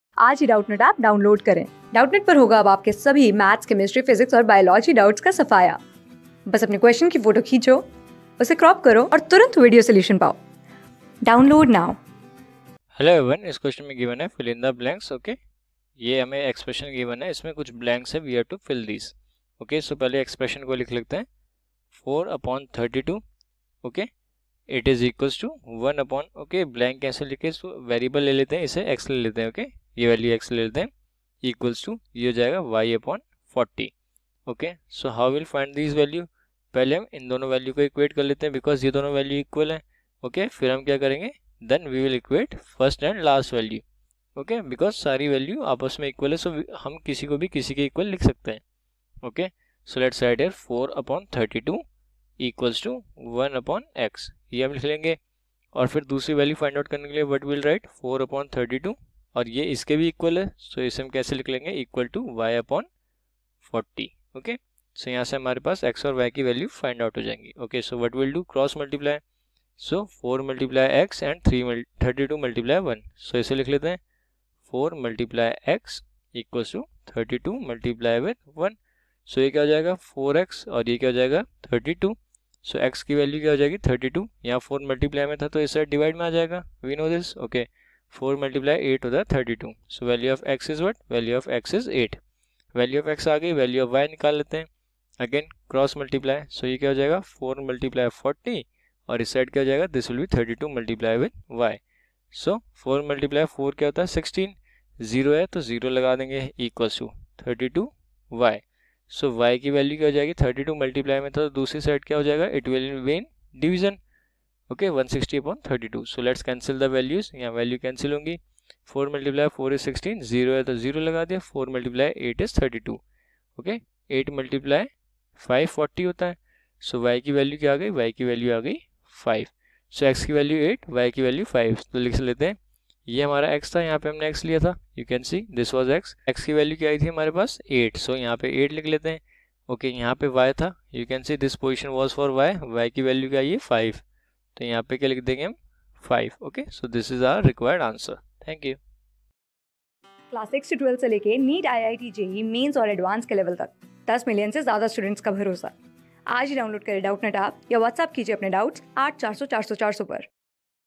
Fill in the blanks : 4/32 = 1/(square) = (square)/(40) | CLASS 6 | RATIO AND PROPORTION | MATHS...
Fill in the blanks : 4/32 = 1/(square) = (square)/(40)
Class: 6
Subject: MATHS
Chapter: RATIO AND PROPORTION
Board:FOUNDATION

👉 Have Any Query? Ask Us.

Our Telegram Pages:
class 6
6 class ka math syllabus